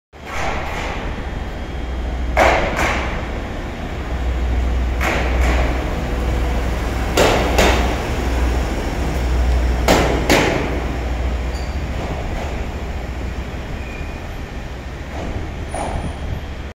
GV-E400系 T113編成 奥羽本線普通列車東能代行の送り込み回送が秋田駅4番線に入線するシーン
10:42秋田